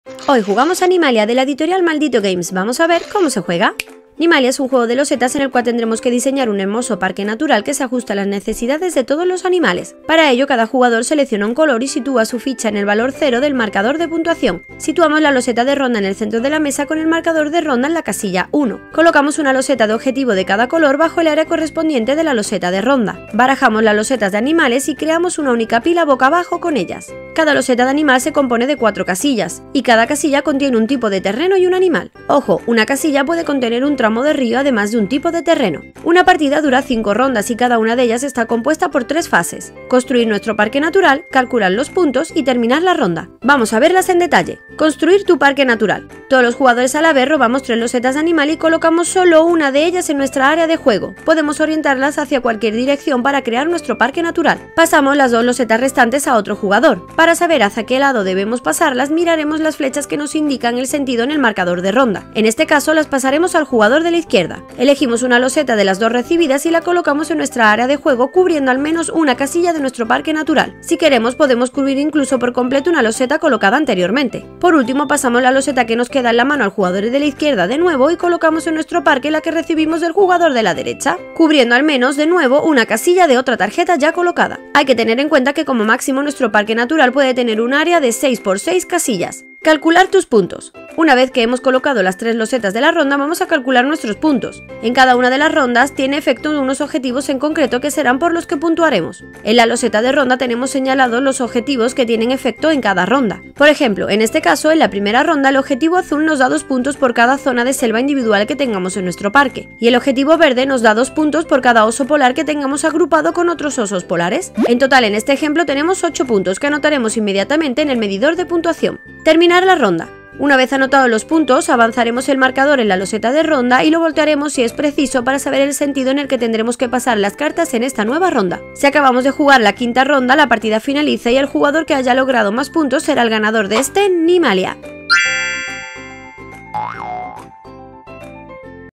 Nimalia - Cómo se juega - Reseña rápida - Juego de mesa
Hoy jugamos a Nimalia
Editorial: @malditogames
Edad: +10
Jugadores: 2-4
Trabajamos: Atención sostenida, percepción visual, toma de decisiones, flexibilidad cognitiva, resolución de problemas…

Nimalia es un juego de losetas en el que tendremos que crear el más hermoso Parque natural que se ajuste a las necesidades de todos los animales para que puedan convivir en armonía.

Un juego de reglas muy sencillas que se desarrolla en un total de 5 rondas, pero que nos hará 🤯 la cabeza para intentar conseguir la mayor cantidad de puntos completando los objetivos activos en cada ronda.

Contiene 11 losetas de objetivo a doble cara que nos darán mucha variabilidad al juego y 60 losetas de animales que harán que cada partida sea diferente.

Juego pequeño, sencillo y sesudo… nos tiene ❤️ ( ya sabes que tenemos debilidad por este tipo de juegos 🫶🏼)

¿Te gustan este tipo de juegos pequeñitos pero matones? Te leo